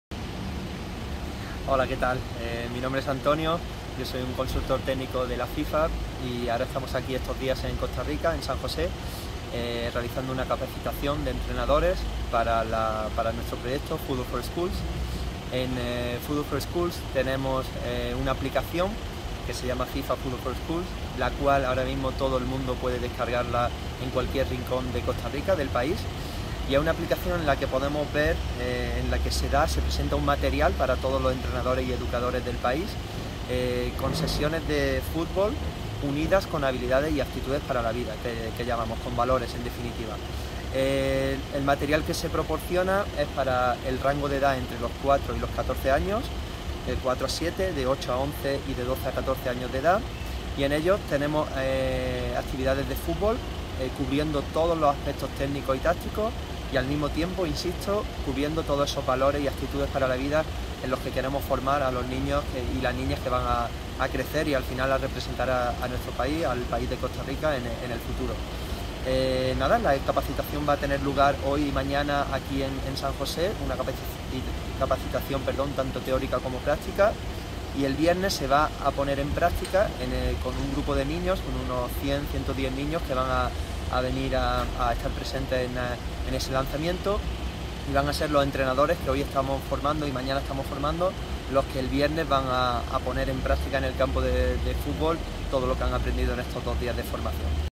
Capacitador de la FIFA, Antonio Buenaño
Antonio Buenaño, consultor técnico de la FIFA, realiza una capacitación a docentes de educación físicas del MEP que impulsarán el futbol en centros educativos.